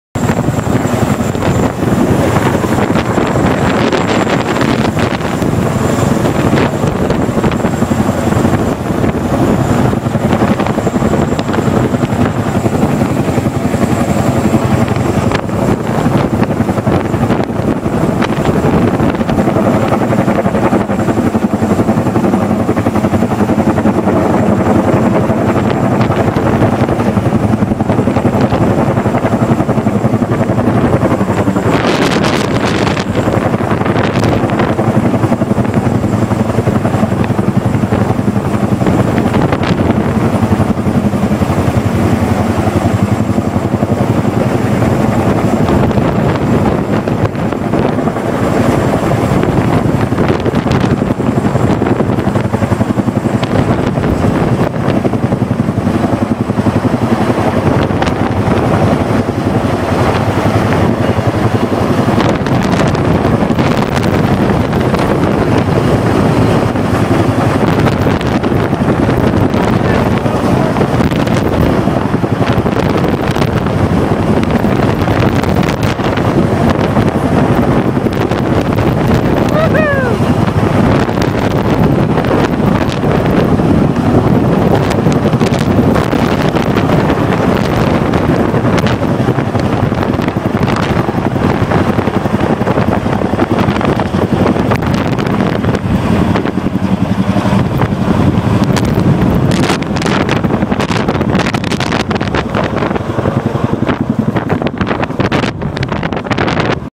Airlifting a M198 howitzer with a CH-47C
Airlifting a US Army M198 howitzer from FOB Shinwar, Afghanistan back to Combat Outpost Garcia 23 kilometers away utilizing a Canadian CH-47C Chinook helicopter. - December 2011 -